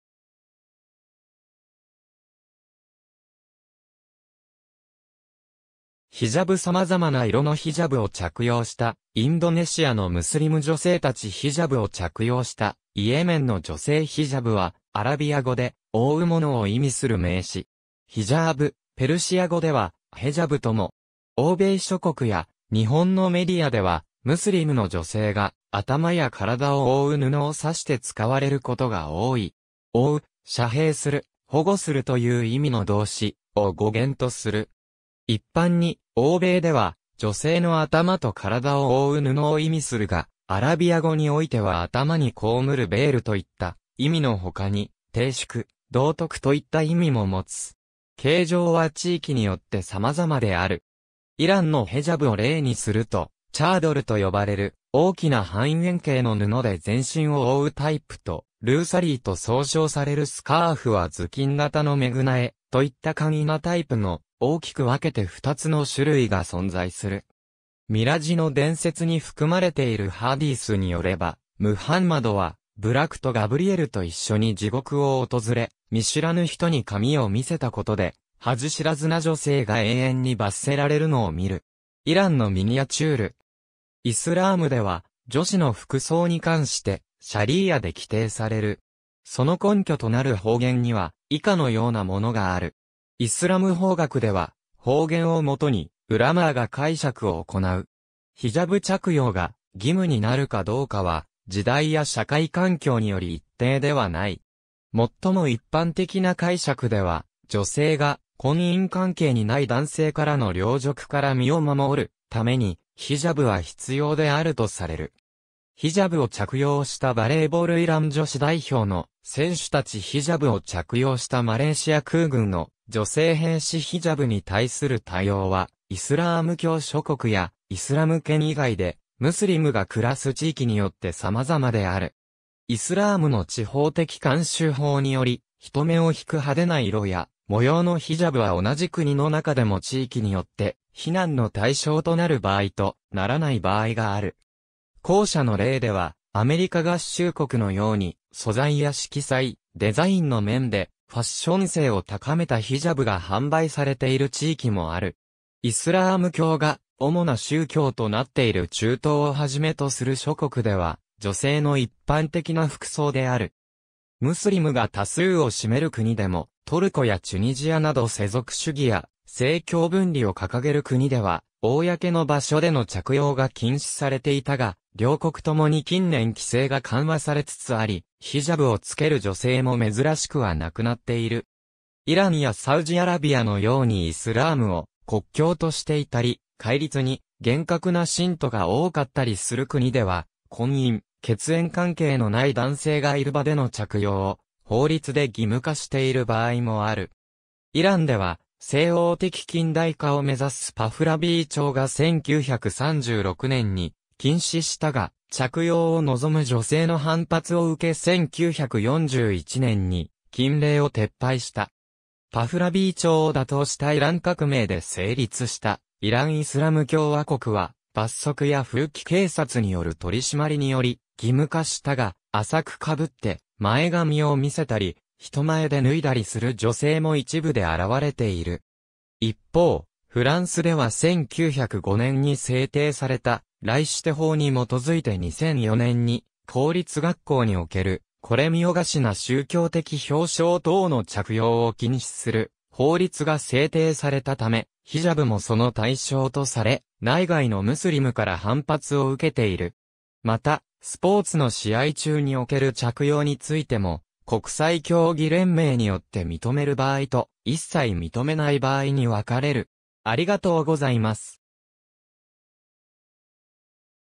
ヒジャブ
ヒジャブ 様々な色のヒジャブを着用したインドネシアのムスリム女性たち ヒジャブを着用したイエメンの女性 ヒジャブ（アラビア語：حجاب. 英語: Hijab, ħijāb）は、アラビア語で「覆うもの」を意味する名詞。
ヒジャーブ、ペルシア語ではヘジャブとも。
欧米諸国や、日本のメディアではムスリム（イスラーム教徒）の女性が頭や身体を覆う布を指して使われることが多い。
「覆う」「遮蔽する」「保護する」という意味の動詞「حجب」を語源とする。
一般に欧米では女性の頭と体を覆う布を意味するが、アラビア語においては頭に被るベールといった意味のほかに「貞淑」「道徳」といった意味も持つ。
形状は地域によって様々である。
イランのヘジャブを例にすると、チャードルと呼ばれる大きな半円形の布で全身を覆うタイプと、ルーサリーと総称されるスカーフは頭巾型のメグナエといった簡易なタイプの、大きく分けて二つの種類が存在する。
ミラジの伝説に含まれているハディースによれば、ムハンマドはブラクとガブリエルと一緒に地獄を訪れ、見知らぬ人に髪を見せたことで「恥知らずな女性」が永遠に罰せられるのを見る。
イランのミニアチュール（15世紀）。
イスラームでは女子の服装に関してシャリーア（イスラーム法）で規定される。
その根拠となる法源には以下の様なものがある。
イスラム法学では、法源を基にウラマー（イスラーム法学者）が解釈を行う。
ヒジャブ着用が義務になるかどうかは時代や社会環境により一定ではない。
最も一般的な解釈では、「女性が婚姻関係にない男性からの陵辱から身を守るために、ヒジャブは必要である」とされる。
ヒジャブを着用したバレーボールイラン女子代表の選手たち ヒジャブを着用したマレーシア空軍の女性兵士(右) ヒジャブに対する対応はイスラーム教諸国や、イスラム圏以外でムスリムが暮らす地域によって様々である。
イスラームの地方的慣習法（ウルフ）により、人目を引く派手な色や模様のヒジャブは同じ国の中でも地域によって非難の対象となる場合と、ならない場合がある。
後者の例では、アメリカ合衆国のように、素材や色彩、デザインの面でファッション性を高めたヒジャブが販売されている地域もある。
イスラーム教が主な宗教となっている中東を始めとする諸国では、女性の一般的な服装である。
ムスリムが多数を占める国でも、トルコやチュニジアなど世俗主義や政教分離を掲げる国では公の場所での着用が禁止されていたが、両国ともに近年規制が緩和されつつあり、ヒジャブを付ける女性も珍しくはなくなっている。
イランやサウジアラビアのようにイスラームを国教としていたり、戒律に厳格な信徒が多かったりする国では、婚姻、血縁関係のない男性がいる場での着用を法律で義務化している場合もある。
イランでは西欧的近代化を目指すパフラヴィー朝が1936年に禁止したが、着用を望む女性の反発を受け1941年に禁令を撤廃した。
パフラヴィー朝を打倒したイラン革命で成立したイラン・イスラム共和国は罰則や風紀警察による取り締まりにより義務化したが、浅くかぶって前髪を見せたり、人前で脱いだりする女性も一部で現れている。
一方、フランスでは1905年に制定されたライシテ（政教分離）法に基づいて2004年に公立学校における「これみよがし」な宗教的標章等の着用を禁止する法律が制定されたため、ヒジャブもその対象とされ、内外のムスリムから反発を受けている。
また、スポーツの試合中における着用についても、国際競技連盟によって認める場合と一切認めない場合に分かれる。